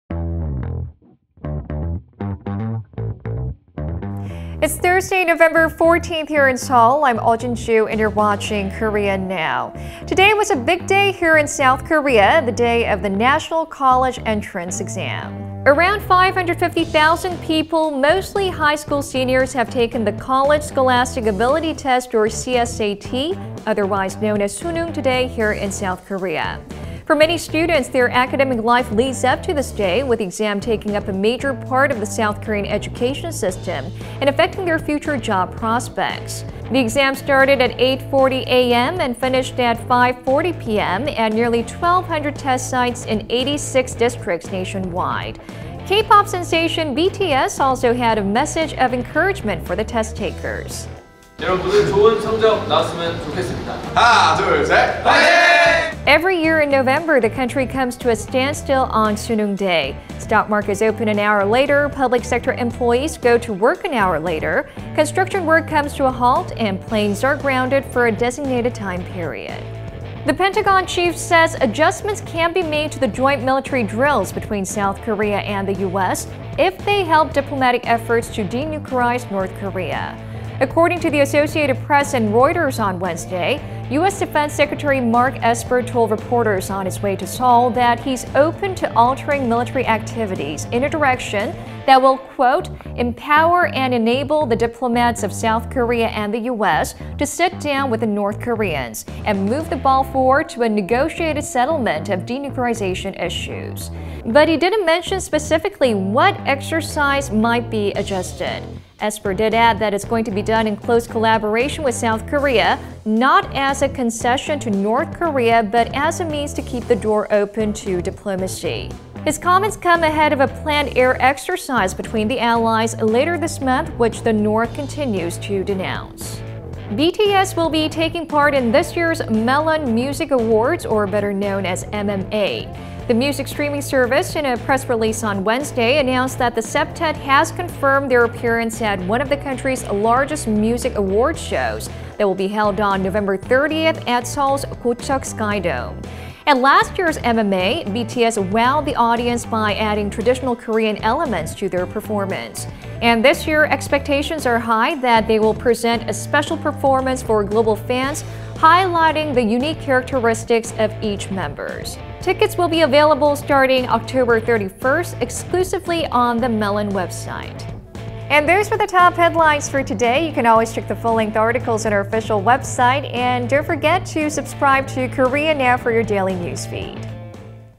550,000 Koreans take national college entrance exam / BTS to perform at Melon Music Awards 2019
1. S. Korea holds national college entrance exam 00:16

2. U.S. defense secretary open to altering military activity in S. Korea: reports 01:10

3. BTS to perform at Melon Music Awards 2019 02:07